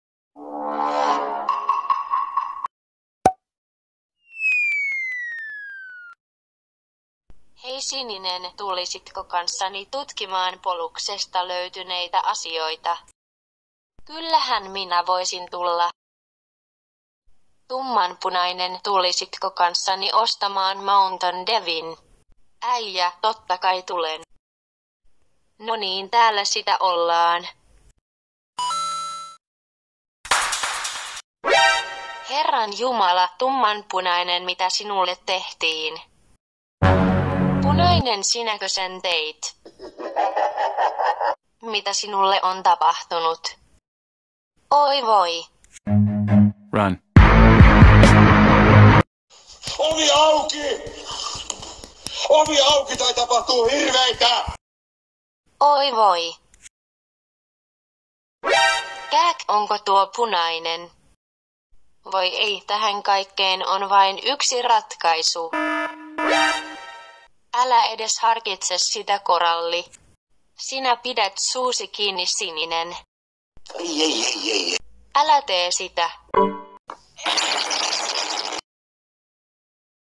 MITÄ TAPAHTUI MIRA HQ:SSA? ||Among Us Parodia
Jatketaan Among Us Parodia sarjaa ja tällä kertaa tapahtumat sijoittuu Mira hq taivas kaupunkiin. Toivottavasti pidätte ja muistakaa laittaa tykkäystä jos piditte videosta 🤗